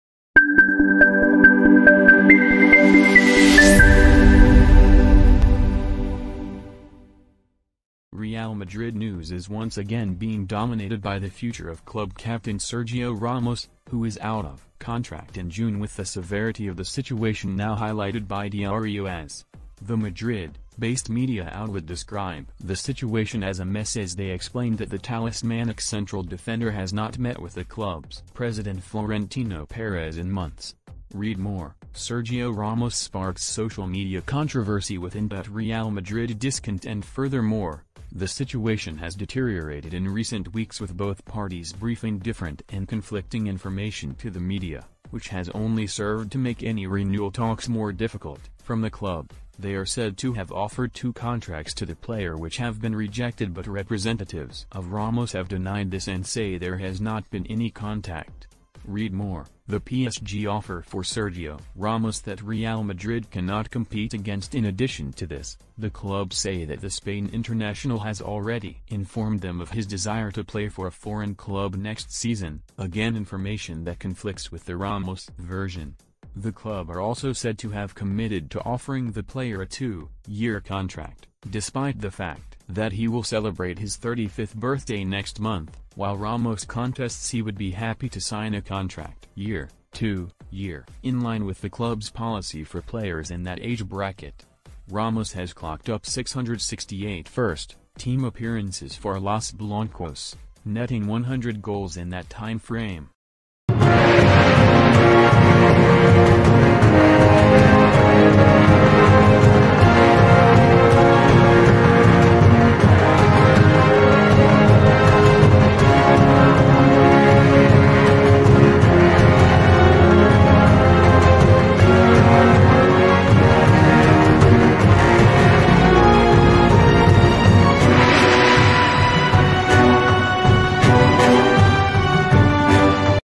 The mess between Real Madrid and Sergio Ramos - news today
Real Madrid news is once again being dominated by the future of club captain Sergio Ramos, who is out of contract in June with the severity of the situation now highlighted by Diario AS. The Madrid-based media outlet describe the situation as a mess as they explain that the talismanic central defender has not met with the club’s presi...